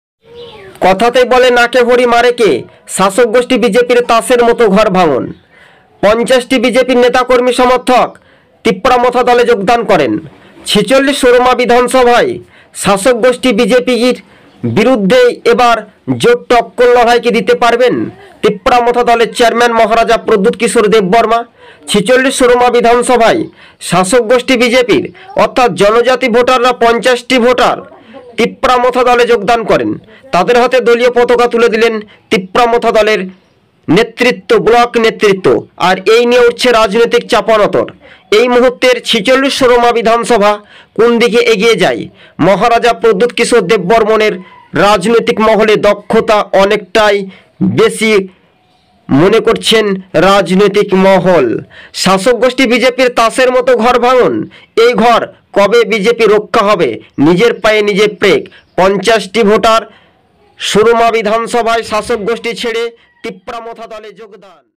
আবারো ত্রিপুরাতে বিজেপির বড় ভাঙ্গন ৫০টি ভোটার বিজেপি ছেড়ে প্রদ্যুত কিশোর দেব বর্মনের দলে যোগদান।
পাহাড়ের  ৪৬ সুরমা বিধানসভায় বিজেপির বড় ভাঙ্গন।।